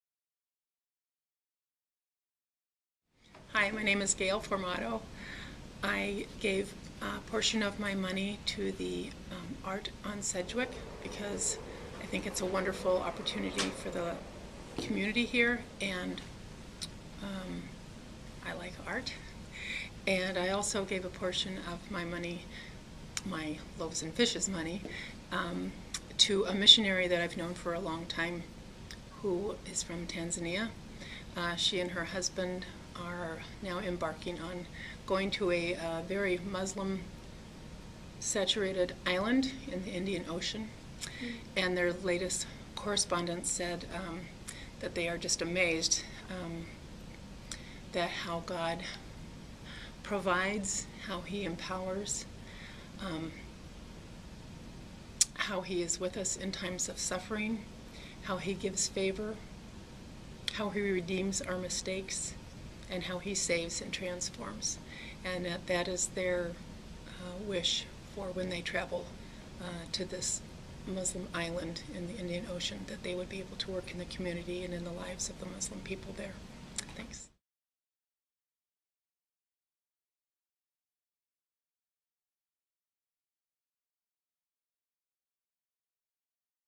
Gale Formato
Discover how this LaSaller felt called to use their Loaves & Fishes $500 tithe check and Let Love Go.

to learn more.

If you are one of the LaSalle members or regular attenders who received a check, join our Facebook group and share your thoughts and experiences with the community